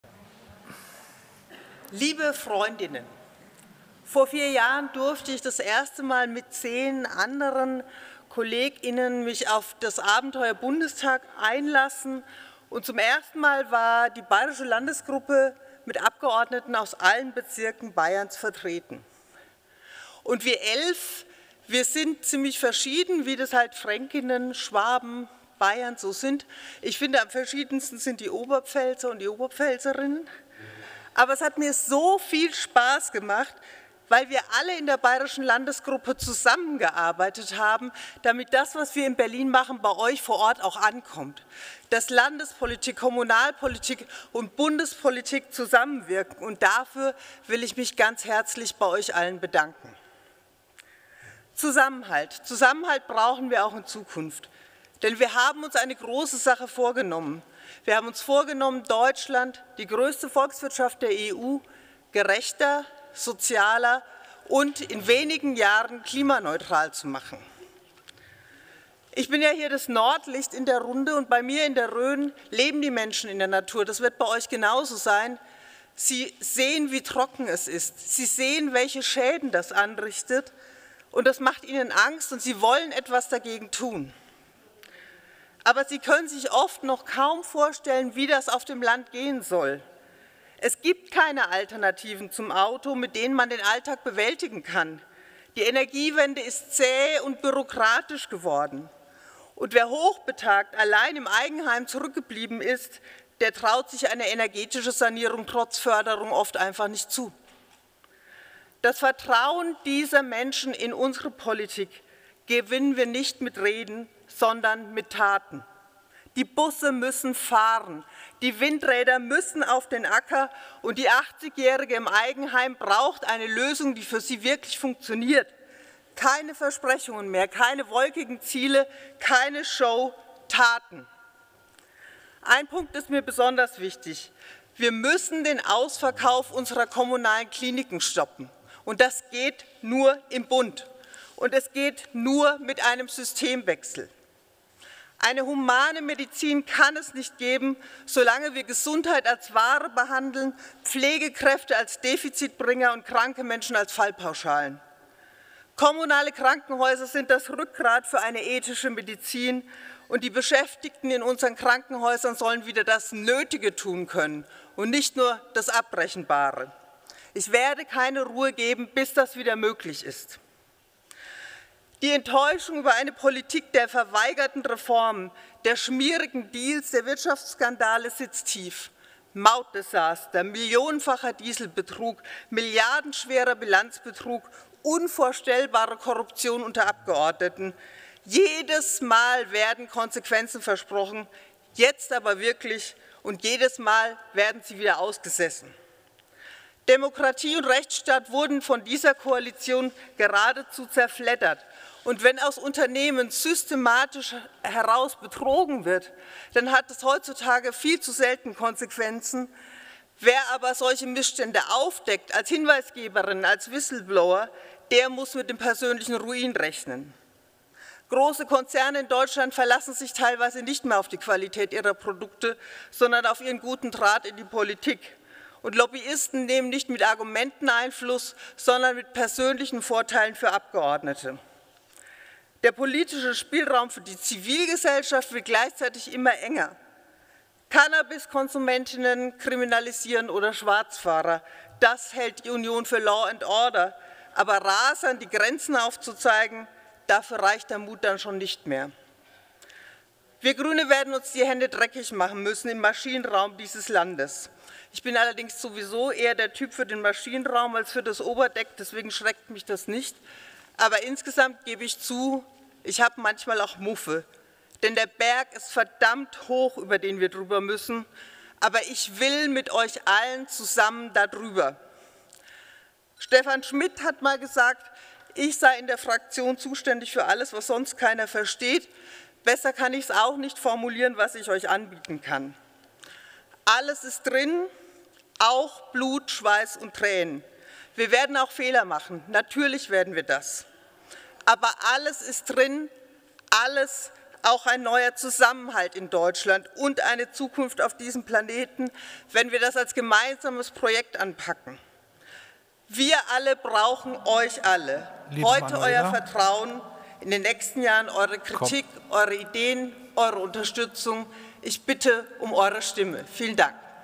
Platz 5 | Manuela Rottmann | Bewerbungsrede zur Bundestagswahl
Mitschnitt vom digitalen Landesparteitag 2021. Alle Infos zur Bundestagswahl und zu unseren Kandidat*innen